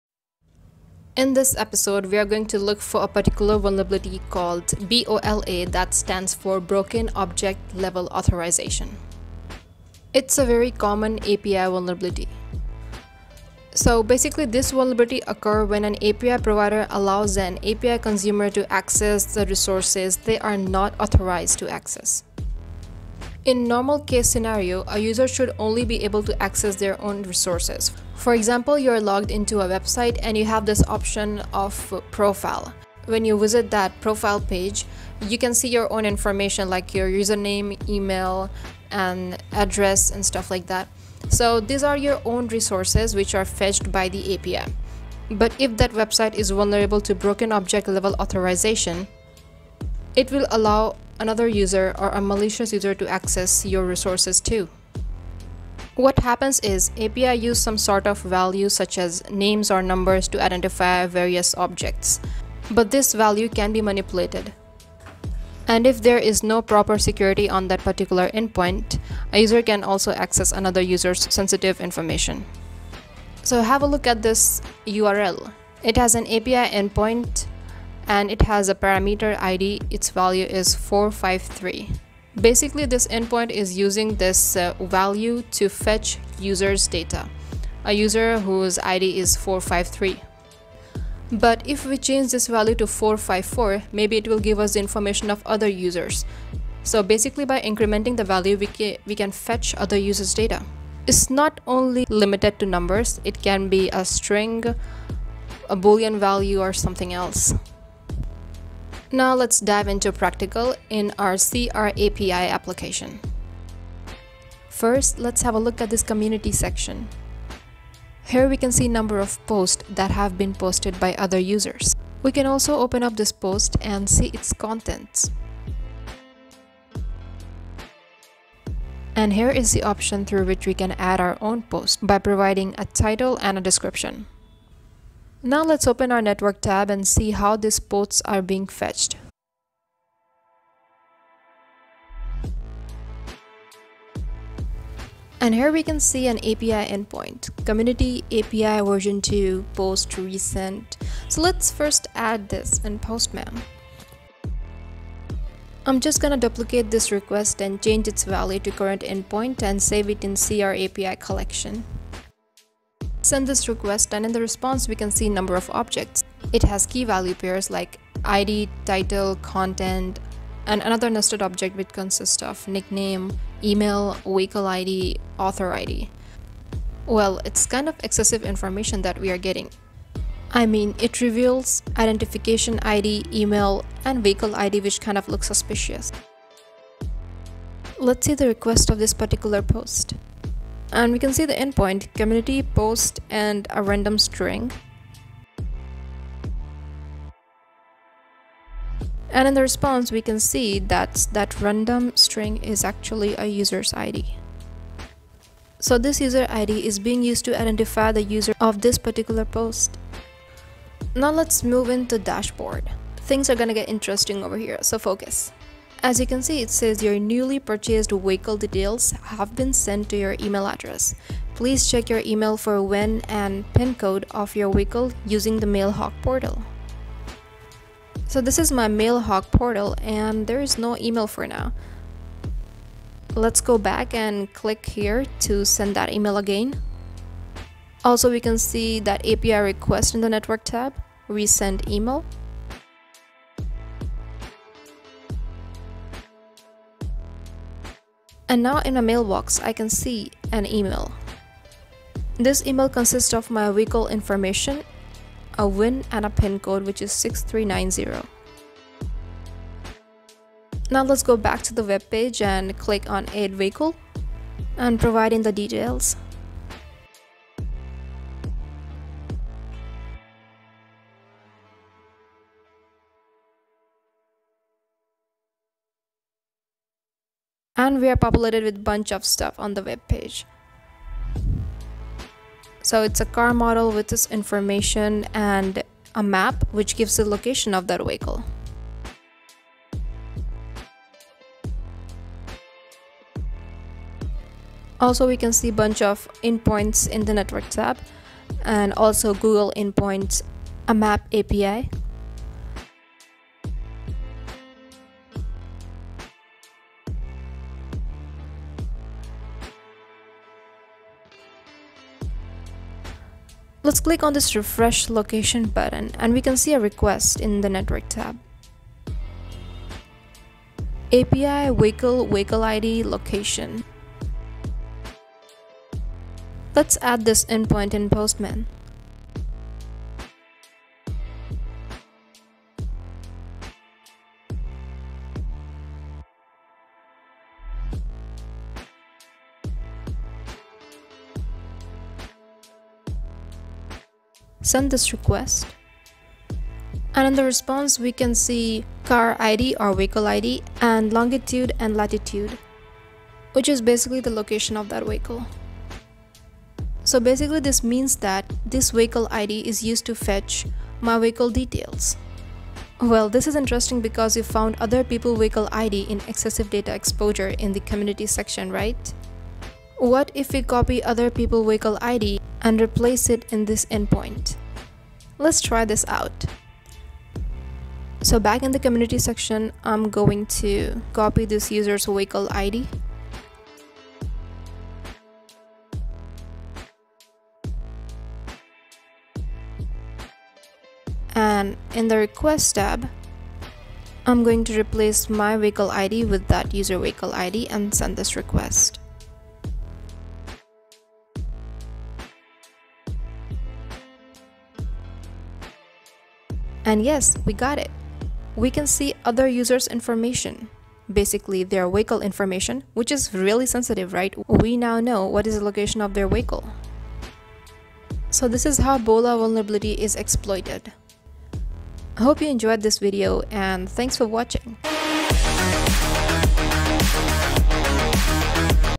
Broken Object Level Authorization | Excessive Data Exposure | crAPI
Part 3 of crAPI Pentesting Series
This video is about Exploiting Broken Object Level Authorization (BOLA)  in API.

What is BOLA? 0:00
Practical: 1:48